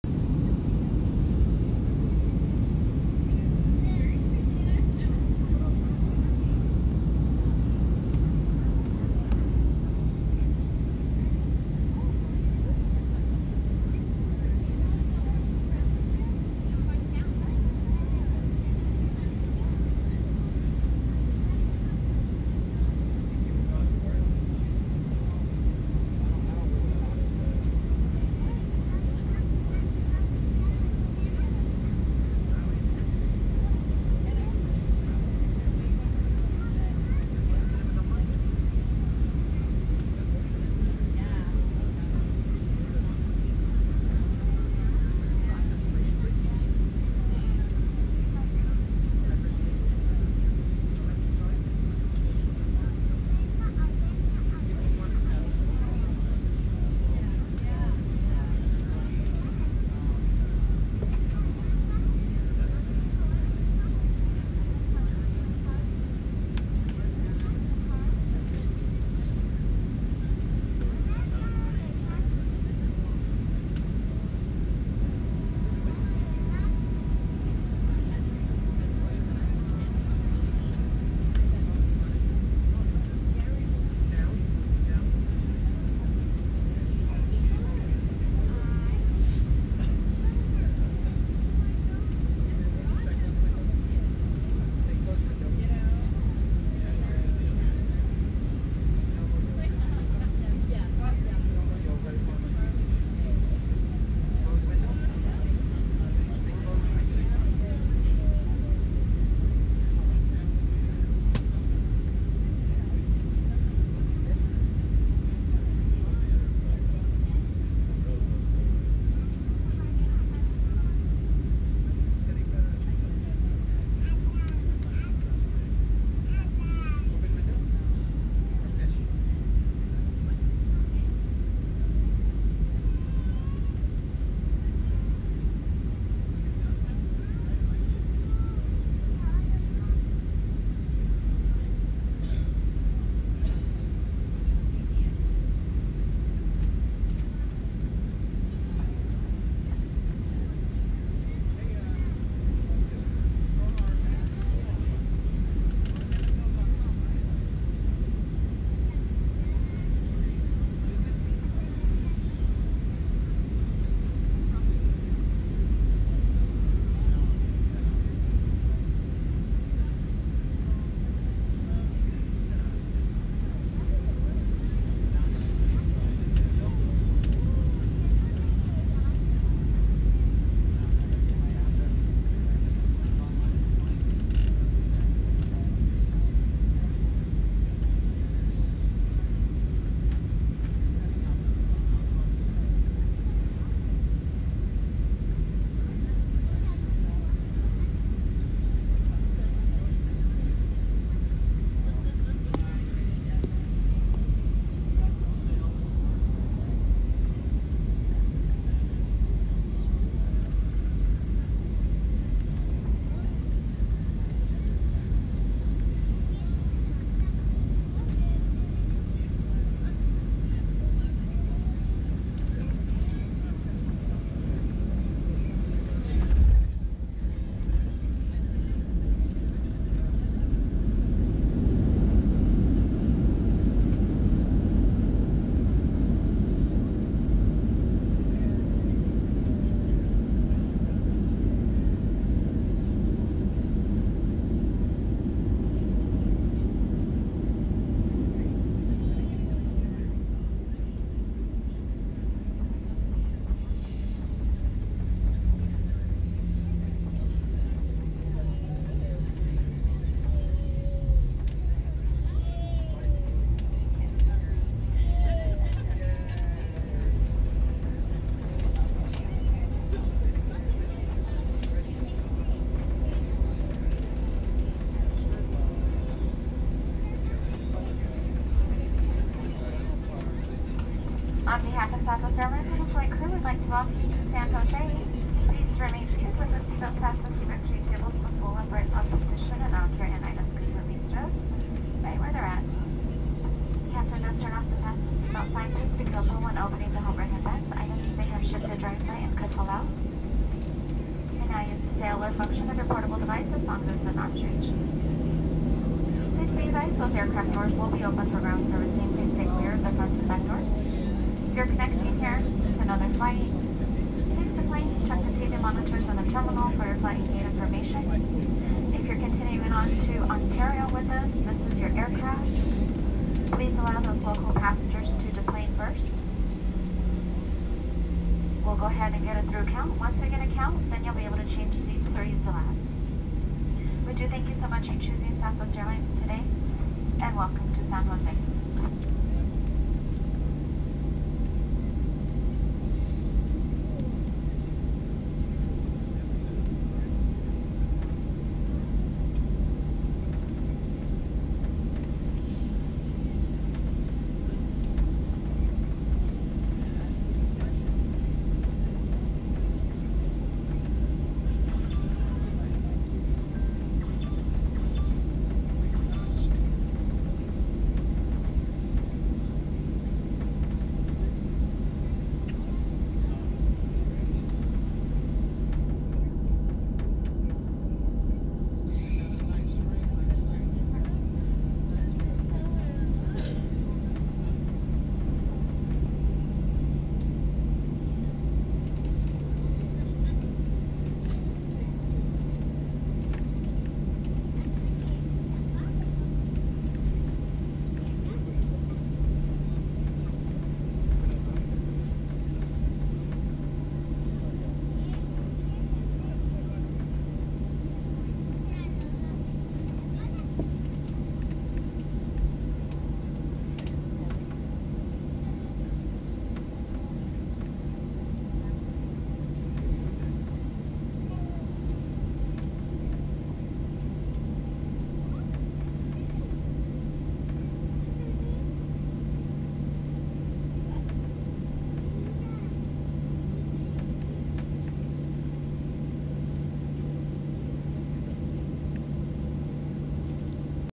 (August 2018) Landing in San Jose, CA (SJC)
[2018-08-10] Here is a direct upload video of my flight landing in San Jose, CA from this past August (2018). As you can see in the video, the airplane takes the southern approach to the airport, which roughly parallels the Guadalupe River and Guadalupe Freeway (CA-87).

This video is completely unedited - please note that my gimbal ran out of power mid-way through the video, so that is why there is abrupt movement.

Shot on iPhone X using Filmic Pro and a Zhiyun Smooth Q gimbal.